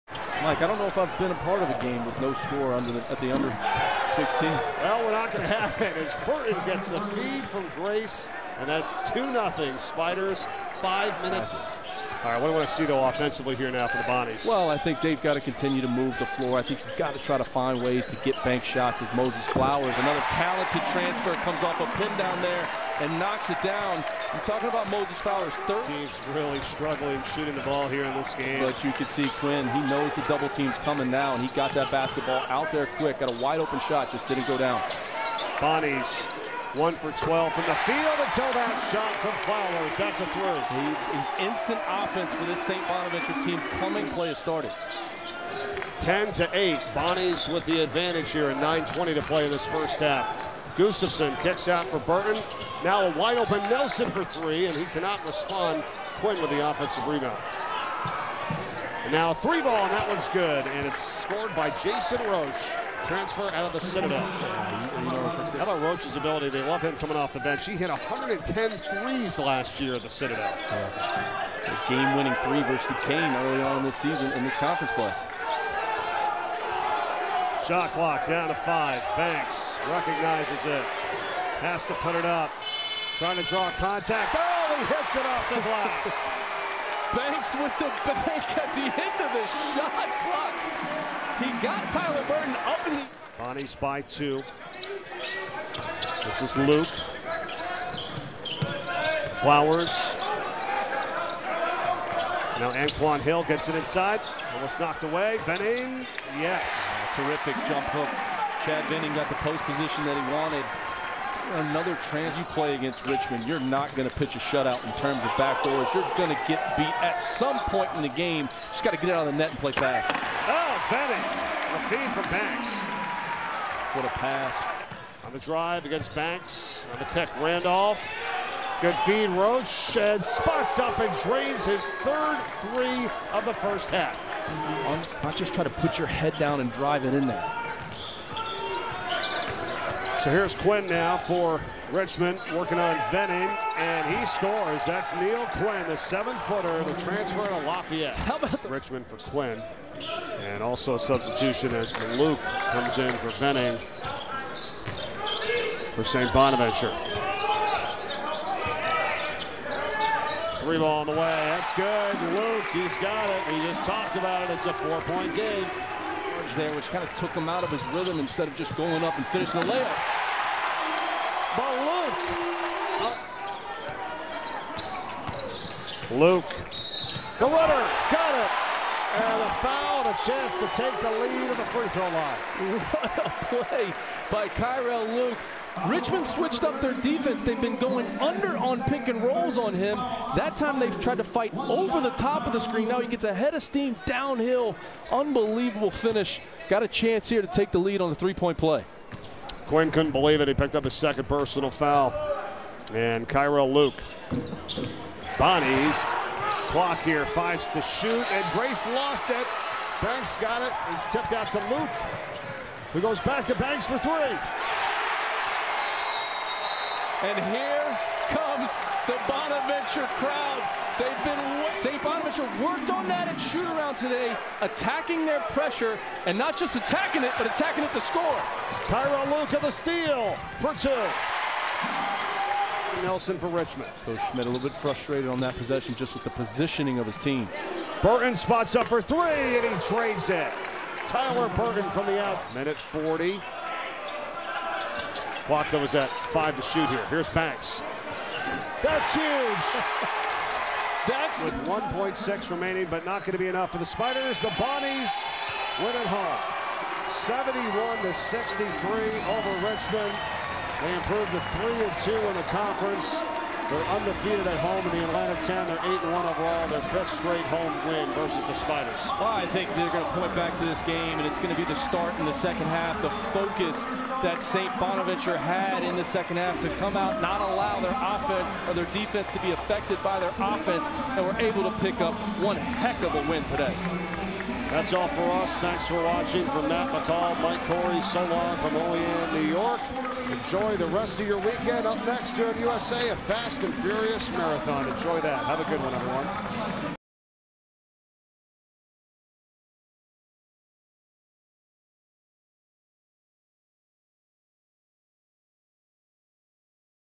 Richmond Spiders vs. St. Bonaventure Bonnies | EXTENDED HIGHLIGHTS | 1/14/23 | NBC Sports
Daryl Banks III and Moses Flowers each scored 20 points and St. Bonaventure overcame a halftime deficit to take down Richmond.